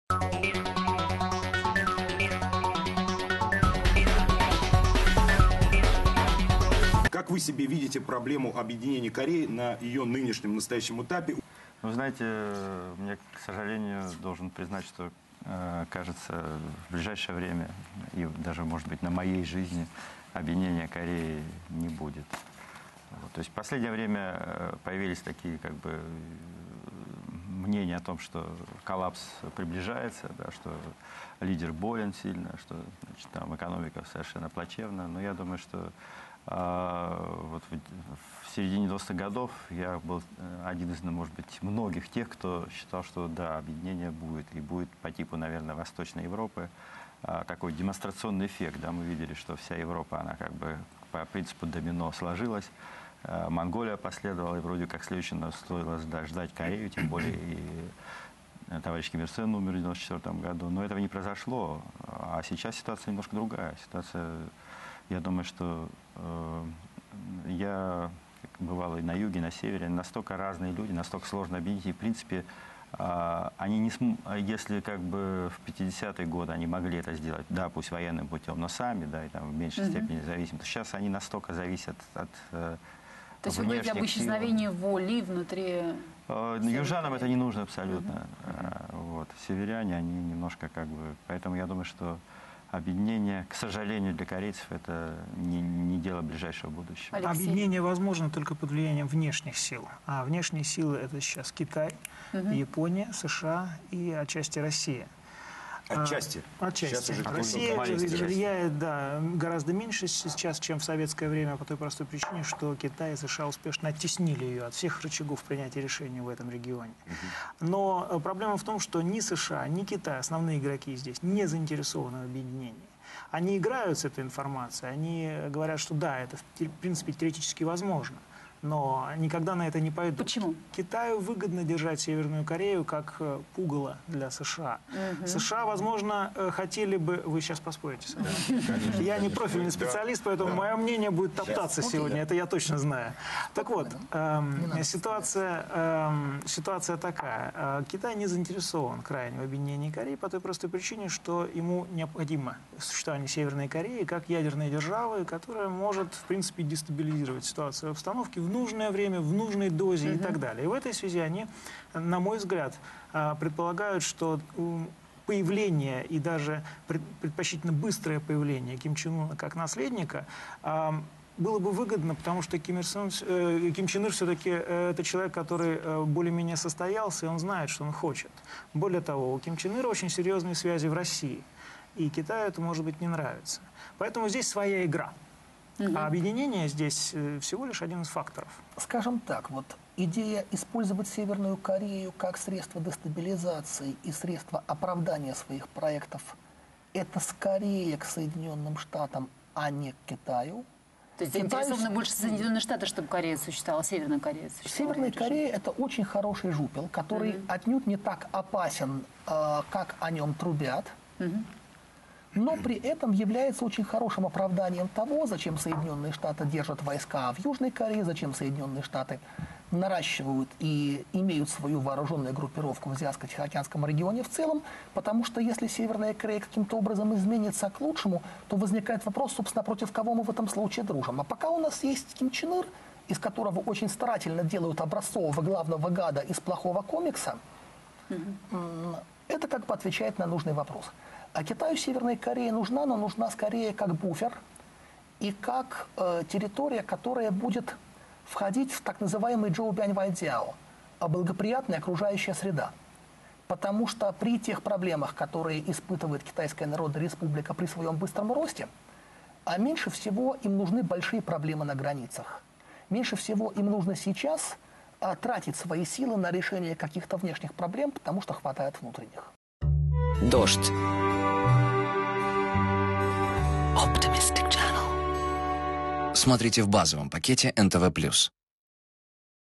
Объединения Кореи не будет /// ФРАГМЕНТЫ
Гости Ольги Дыховичной и Александра Ф. Скляра директор Центра корееведения МГУ Павел Лешаков,  ведущий научный сотрудник Института Дальнего Востока РАН Констанин Асмолов и глава Центра политической информации Алексей Мухин объясняют, почему объединения Северной и Южной Кореи в ближайшее время ждать не стоит.